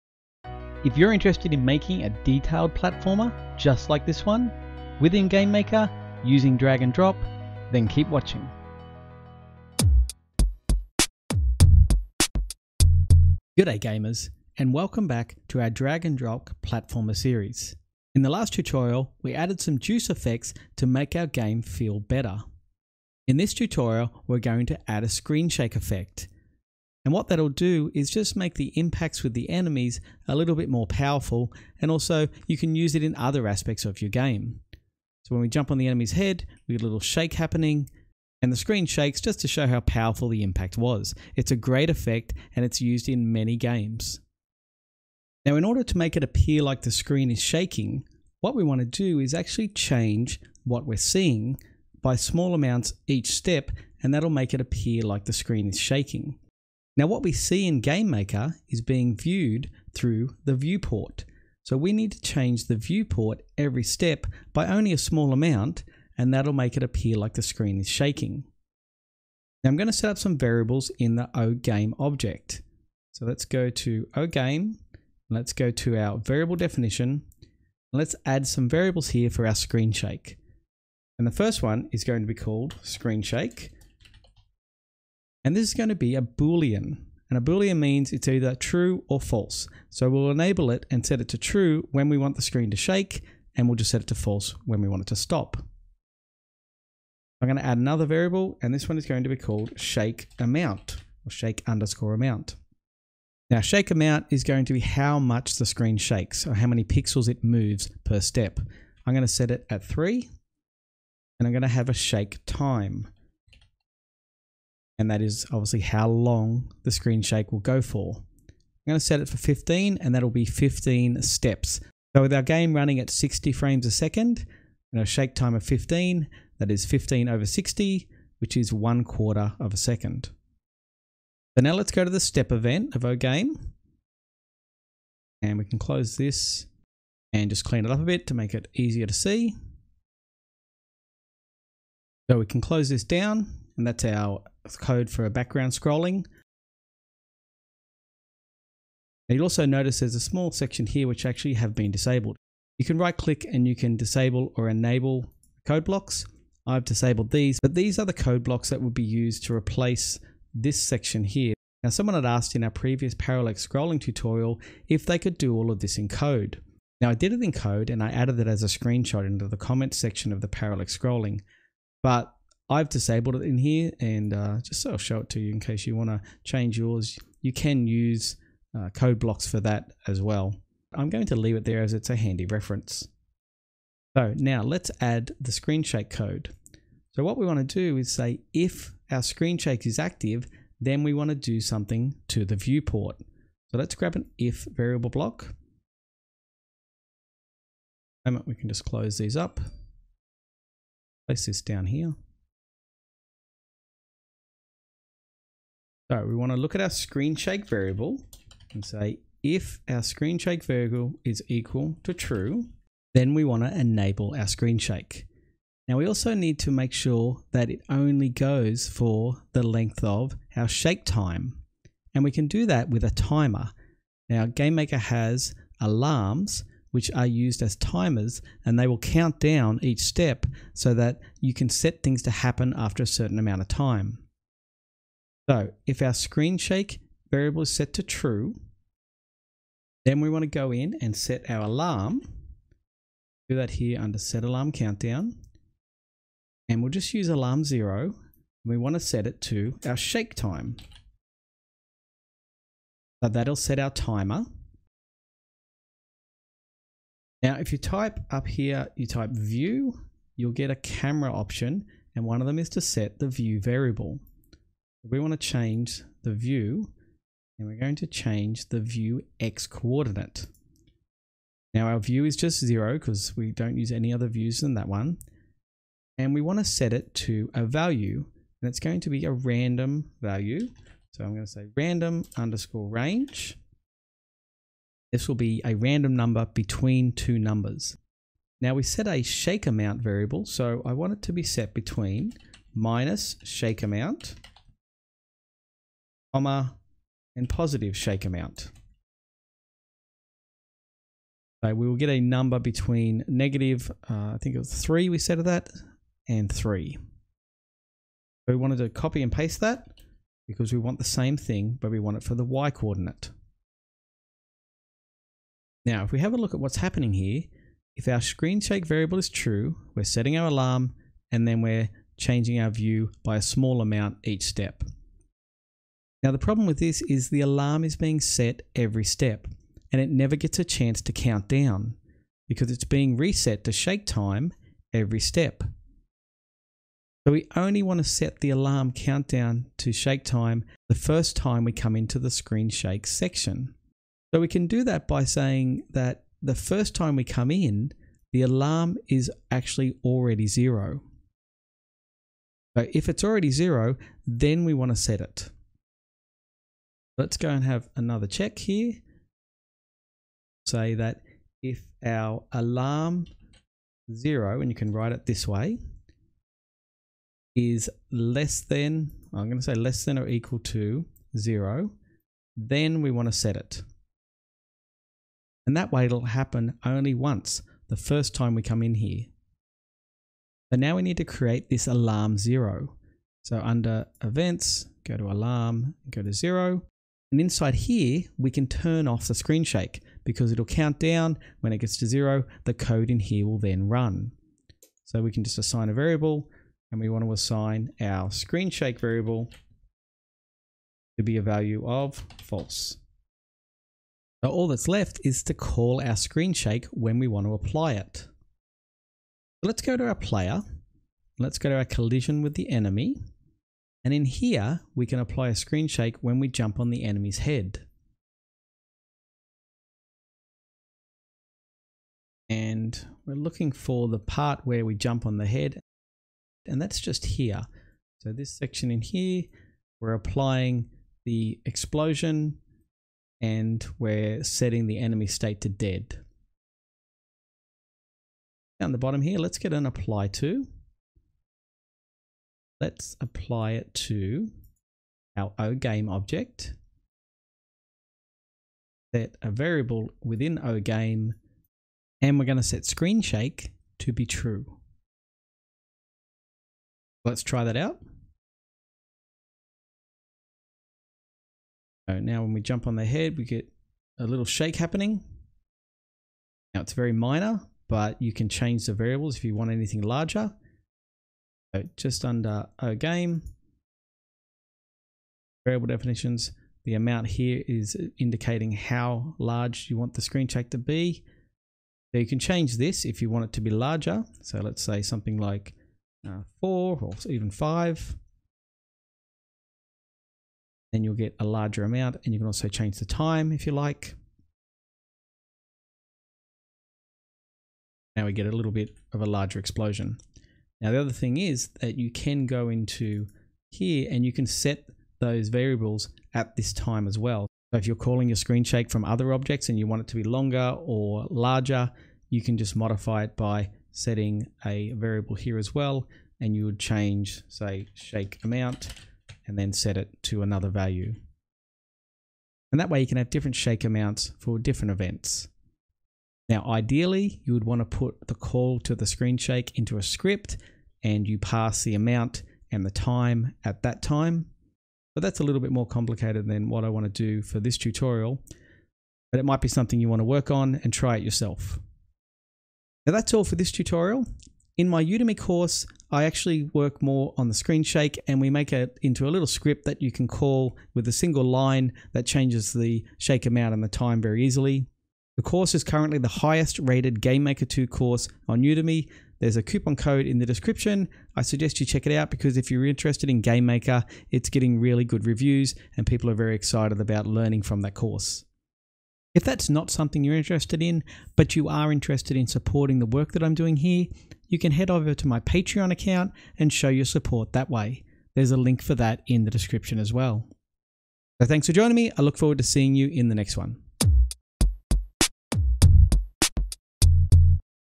Gamemaker DND Platformer Tutorial - #12 Screen Shake
In this series I'll show you how you can use Gamemaker Studio 2's Drag and Drop to create a cool looking platformer.  Some of the things we'll cover in the series include adding tiles to make it look great, adding enemies which we can jump on, and even adding one way platforms.  We also make an animated player character that has an idle, walk and jump animation.

In this tutorial we add a cool screen shake effect when we jump on an enemy, but you can use it anywhere to give your game more impact.

Download and unzip them into a folder, and they will be ready to use in the tutorial.

If you want to be notified of new content, or things related to Gamemaker, sign up to my newsletter

Gamemaker Studio 2 is made by Yoyo Games.